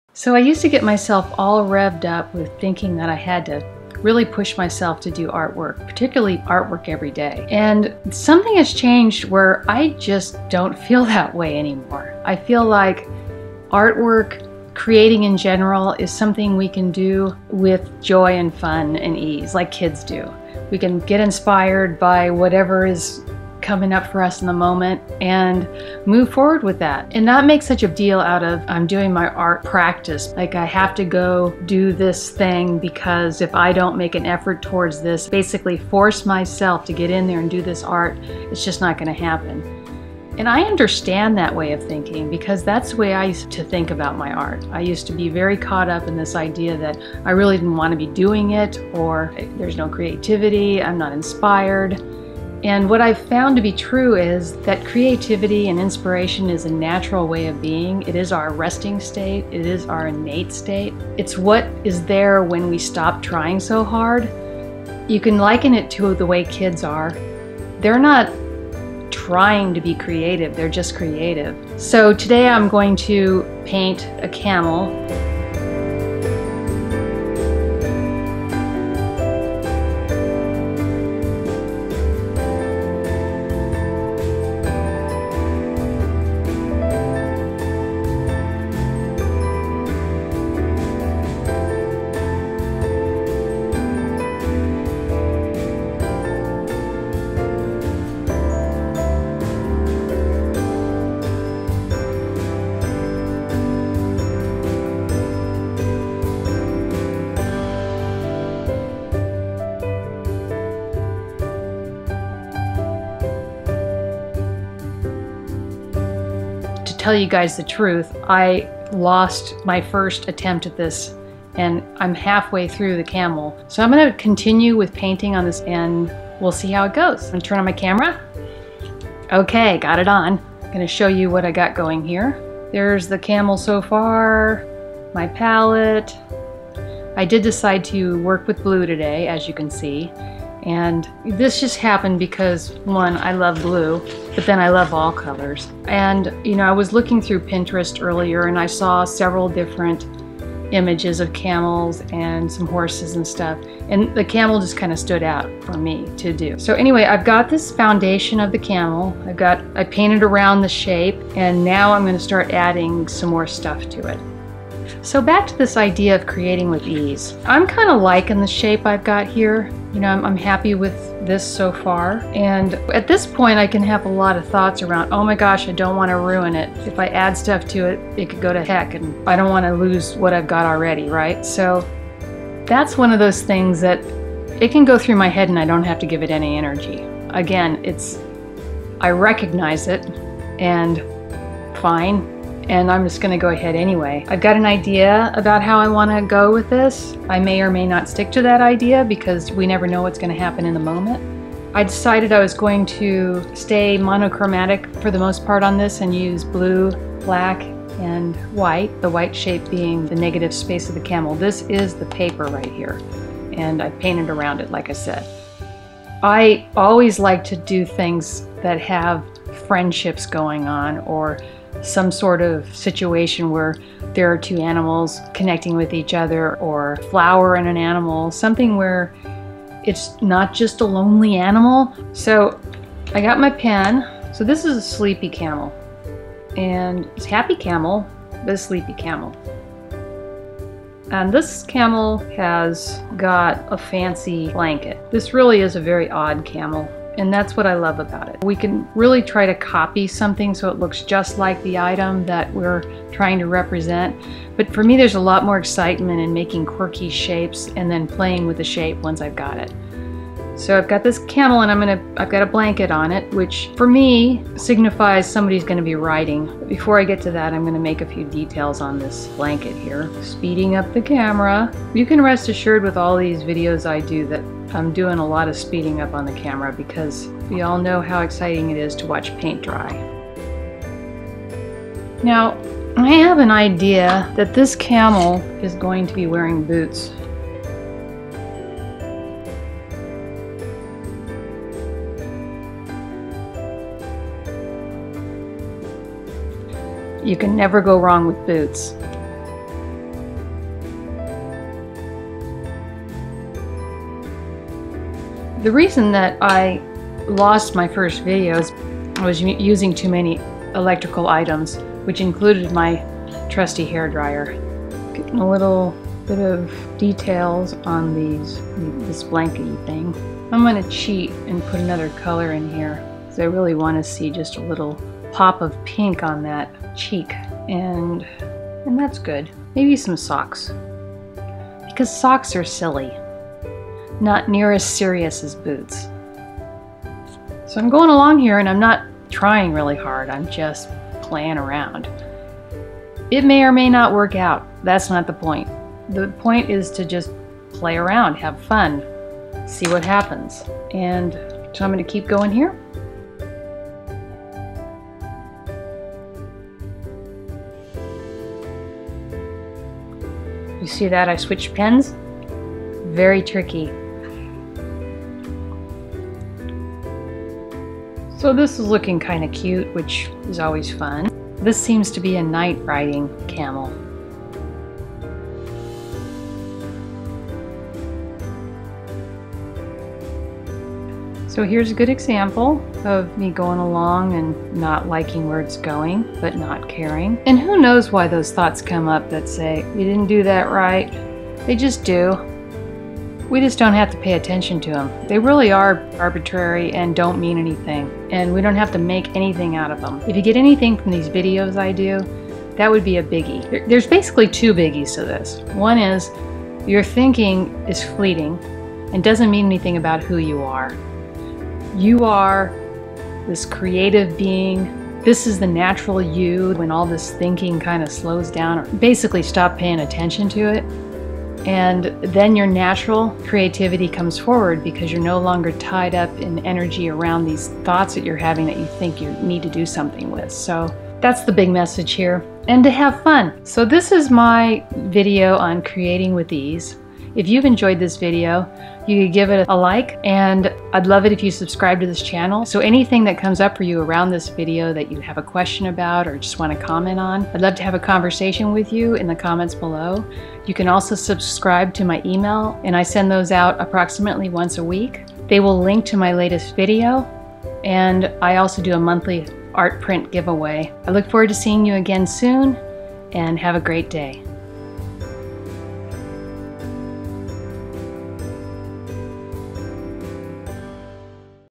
How To Create With Ease
In this video I talk about creating with ease!  Creativity is our natural state, not a chore to do.  I show through conversation and a watercolor painting demonstration that we don't have to be distracted by our thinking/feelings.  We can keep moving forward in the moment regardless of thoughts we may have to the contrary.

I have a monthly print giveaway from my Etsy shop for email subscribers.

Paper Stonehenge 7x7" Cotton
MT Washi Tape
Black Pen Med Staedtler Permanent Lumocolor F
Black Pen Fine Staedtler Permanent Lumocolor S
White Pen Fine Uni Ball Signo Broad
White Pen Acrylic Thick Molotow One4all Acrylic 227HS